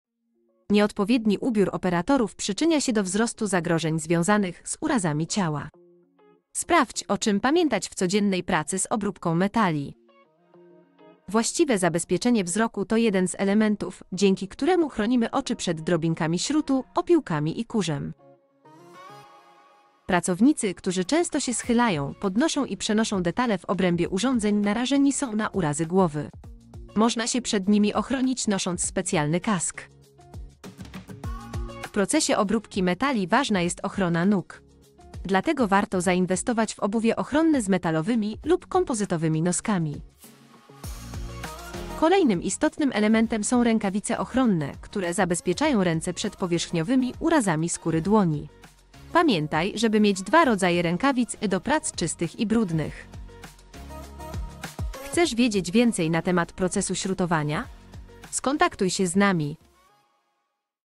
BHP - 4 rzeczy, o których musisz pamiętać podczas obróbki metali ⛑
Odzież i obuwie ochronne są niezbędne do zachowania bezpieczeństwa w zakładzie produkcyjnym. Sprawdź, o co musisz zadbać podczas obróbki powierzchni!